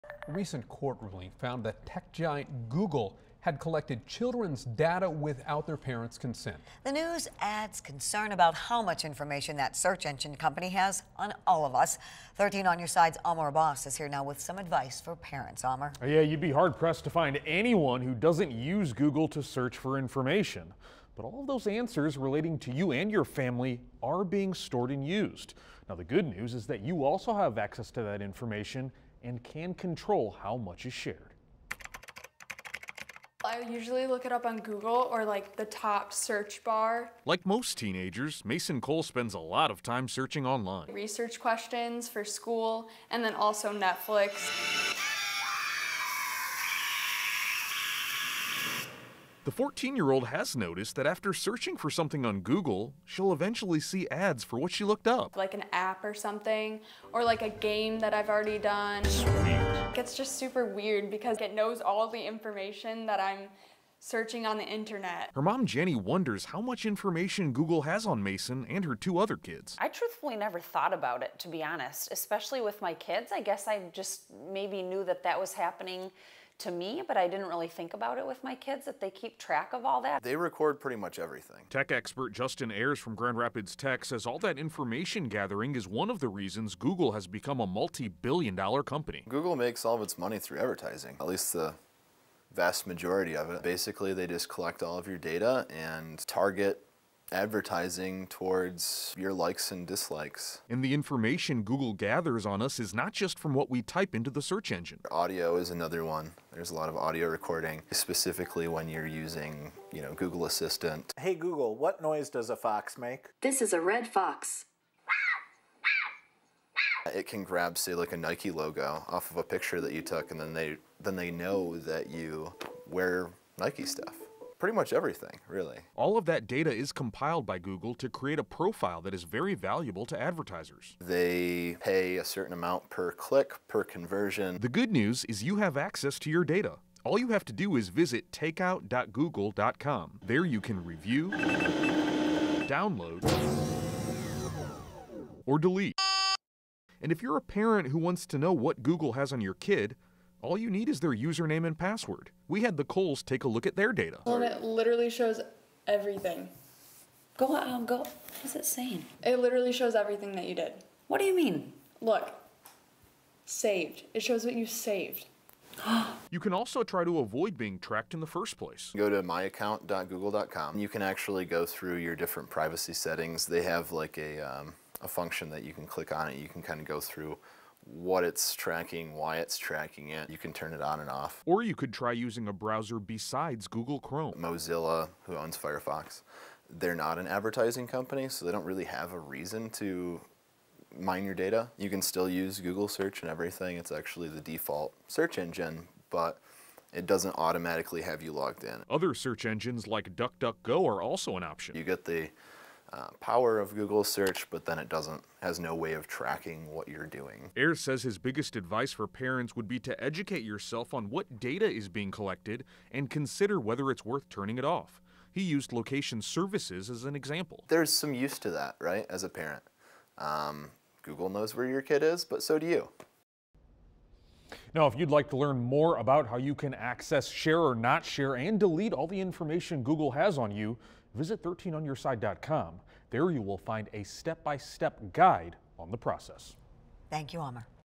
Erasing your Google footprint
A recent court ruling found that tech giant Google had collected children's data without their parents consent.